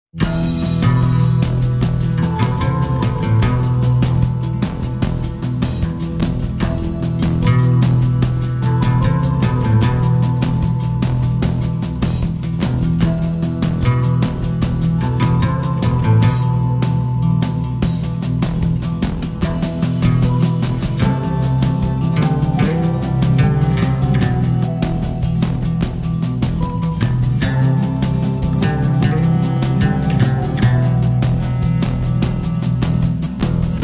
Ultima problemă a patronului Fermei Dacilor |Libertatea
Află detalii: Ultima problemă a patronului Fermei Dacilor. Motivul pentru care șoferii de taxi refuză să-i accepte cursele. „Generează niște situații neplăcute”